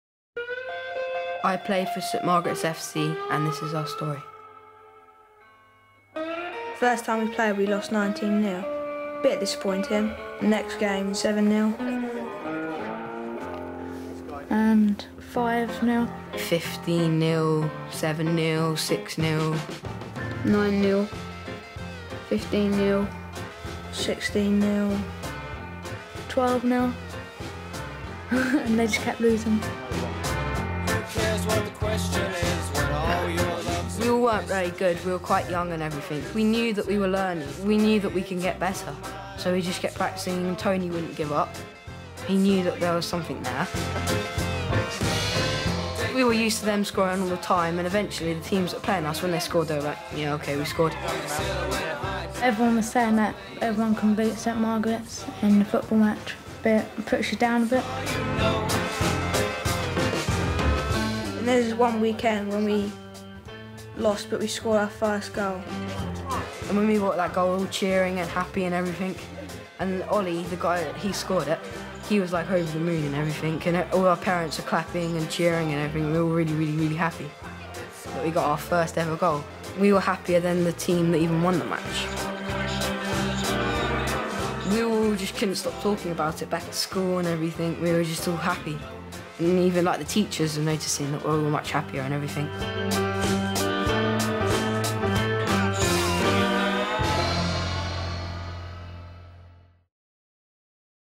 MATCH Magazine - Adidas & St. Margaret's
St. Margaret's Under-13s are the new stars of the Adidas 'Impossible is Nothing' TV ad!